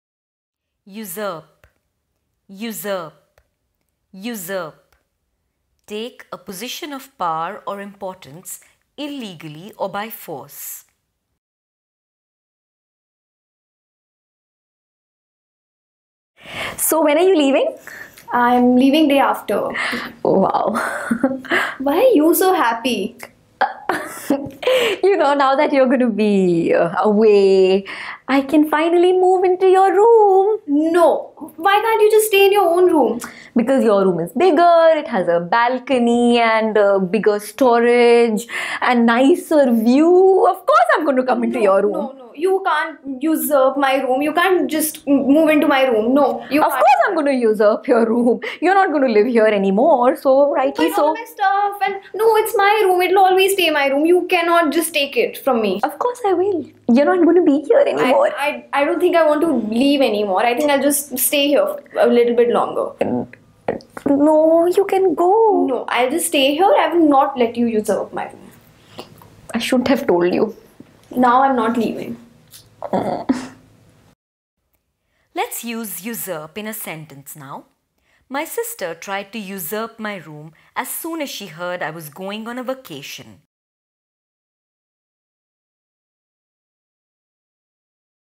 Usurp Meaning | VocabAct | NutSpace
--- MEANING ---

Usurp: take (a position of power or importance) illegally or by force.

--- SENTENCE ---

My sister tried to usurp my room as soon as she heard I was going on a vacation.

replace (verb)
to be used for doing the same job as something that was used before

change (verb)
to replace something with a new or different thing

renew (verb)
to replace something that is old or damaged

supersede (verb)
if one thing supersedes another, it replaces the other thing, especially because it is more modern or more useful

swap (verb)
to replace one thing with another

substitute (verb)
to remove one thing and put something else in its place

give way (phrase)
to be replaced by something, especially something newer or better